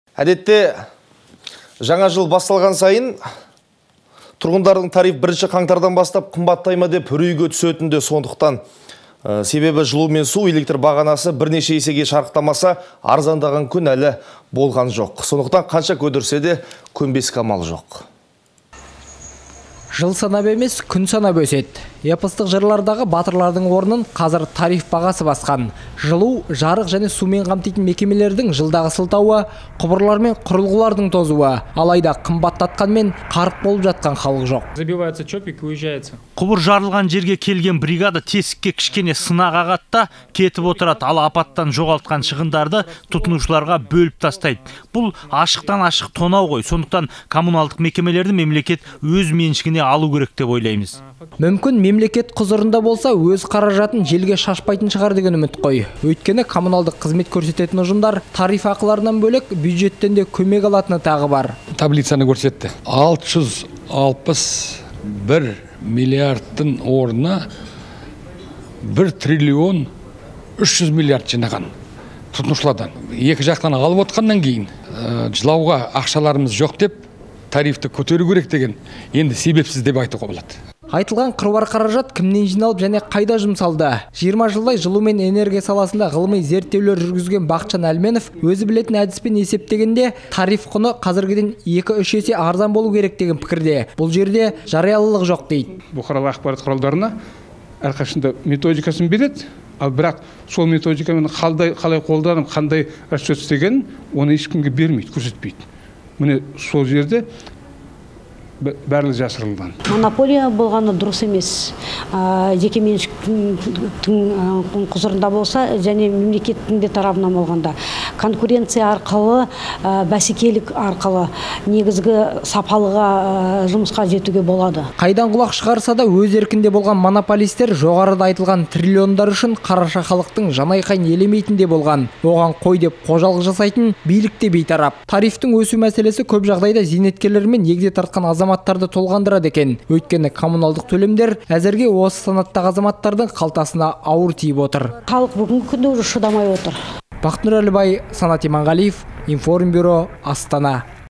Тариф тауқыметі
Жаңа жыл жақындағанда кей азаматтар мерекені ойлап қуанып жатса, бәз біреулер 1 қаңтардан бастап тариф қымбаттай ма деп уайымға бөленеді. Жылу мен су, электр бағасы бірнеше есеге шарықтамаса, арзандаған күн әлі болған жоқ. Сондықтан, қанша көтерсе де көнбеске амал жоқ. Өйткені, әбден басынған монополист мекемелер қит етсе өркениеттен мақұрым қыламын деп қорқытады.